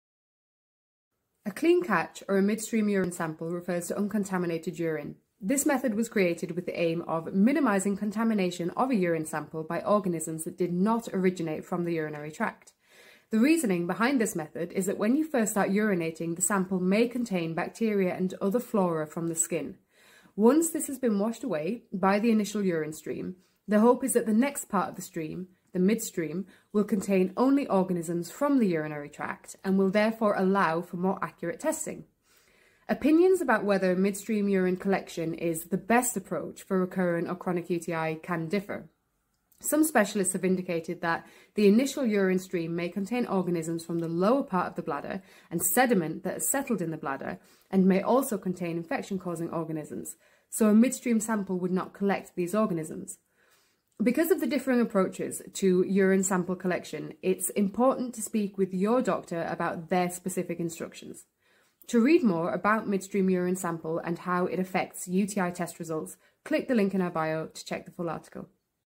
Clean Catch Urine
What is a clean catch or midstream urine sample?

A clean catch, or a midstream urine sample refers to ''uncontaminated urine''. This method was created with the aim of minimising contamination of a urine sample by organisms that did not originate from within the urinary tract.

When you first start urinating, the urine sample may contain bacteria and other flora from the skin. Once this has been washed away by the initial urine stream, the hope is that the next part of the stream - the midstream - will contain only organisms from the urinary tract and will therefore allow for more accurate testing.

Opinions about whether midstream urine collection is the best approach for recurrent or chronic UTI can differ. Some specialists have indicated that the initial urine stream may contain organisms from the lower part of the bladder, and/or sediment that has settled in the bladder and may also contain infection causing organisms. A midstream sample would not collect these organisms. Because of the differing approaches to urine sample collection, it's important to speak with your doctor about their specific instructions.

To read more about midstream urine sample and how it affects UTI test results, click the link to check out the full article

You can find more content from Saoirse @the.wholehealthcoach